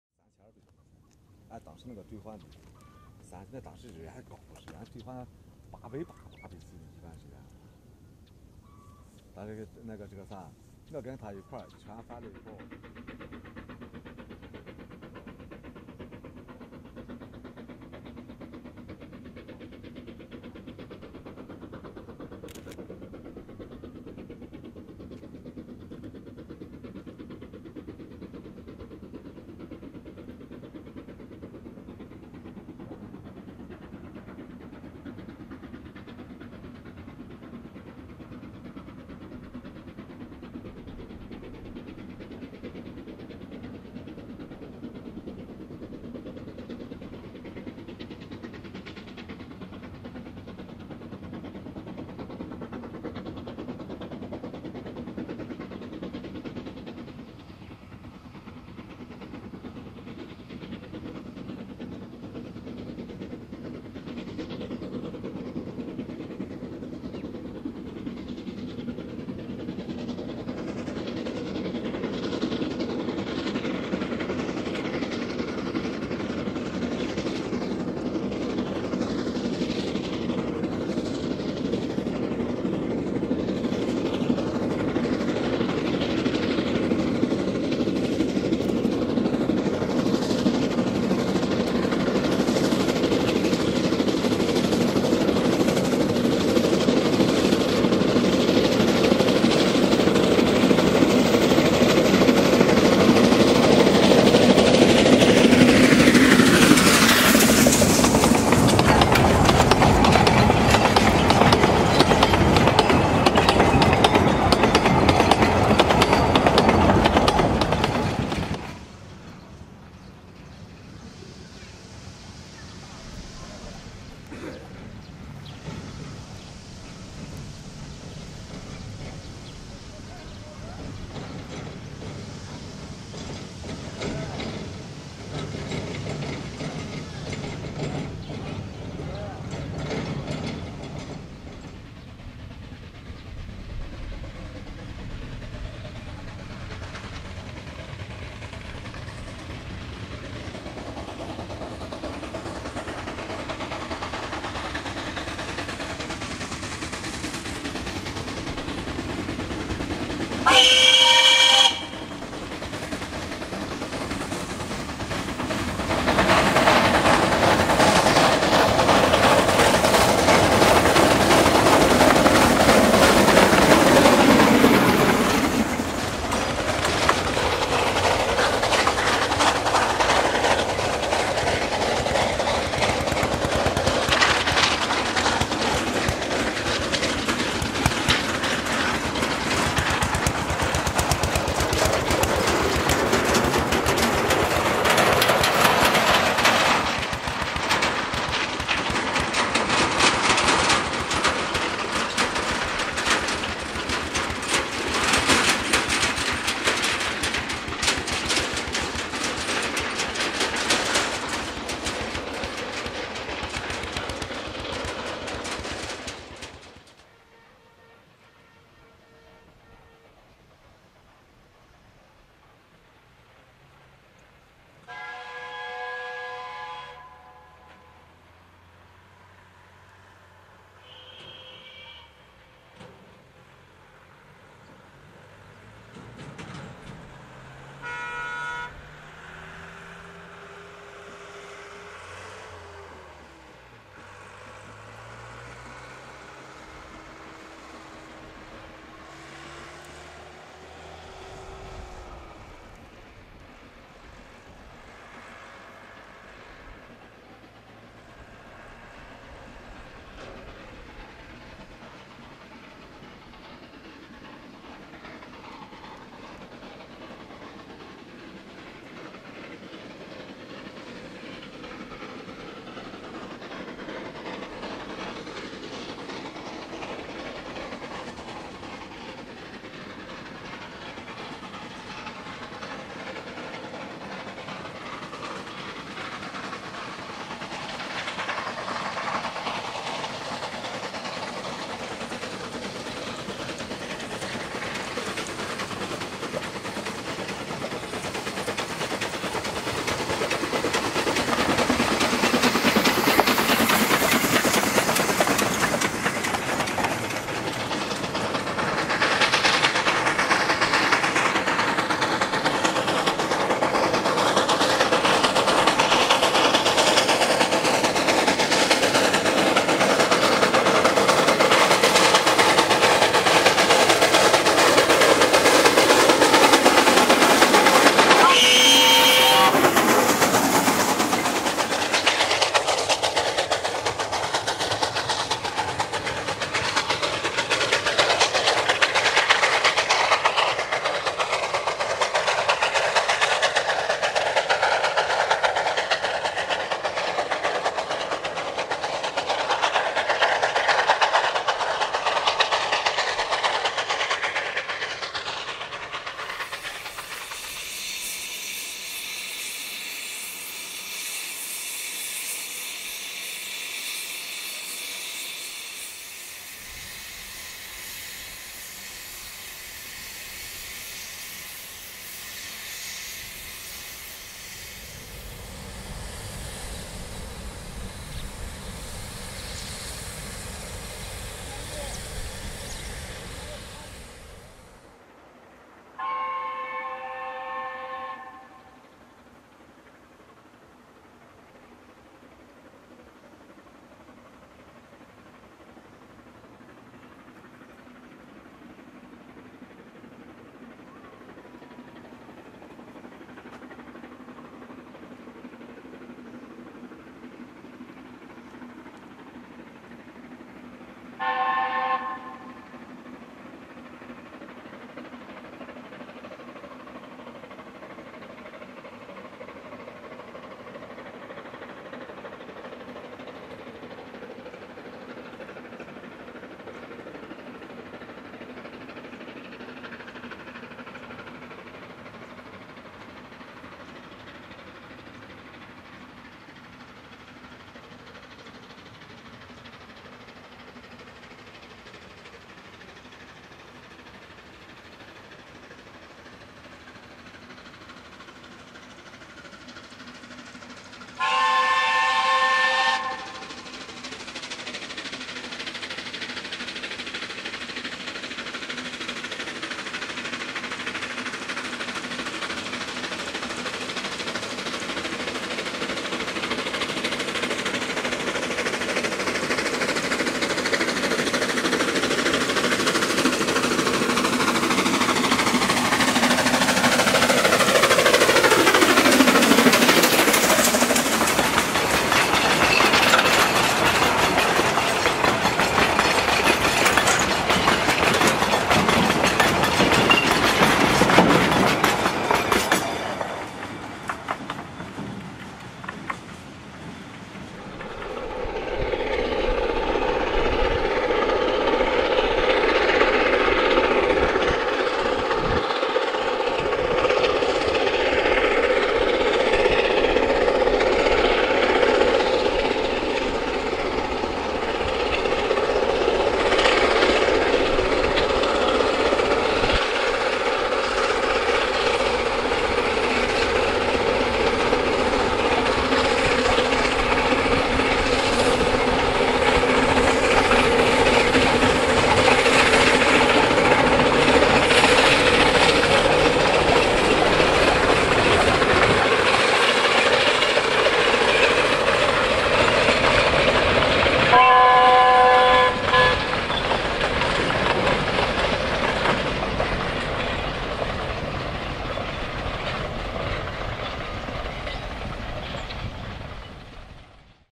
Yinghao coal railway 2 （May,2006）
This line is or was? located Yinghao,  Henan province, China.
Green of trees was really beautiful though smoke was a little.
Note: Almost same video was uploaded on Stage6 DivX before.

河南省の英豪専用線です。現在は休止中（廃止？）のようです。5月はじめというのに、死にそうに暑くてほとんど無煙でしたが、木々の緑がとても綺麗でした。 なお、ほとんど同じビデオをStage6 DivX にアップロードしていました（サイト自体が消滅しています）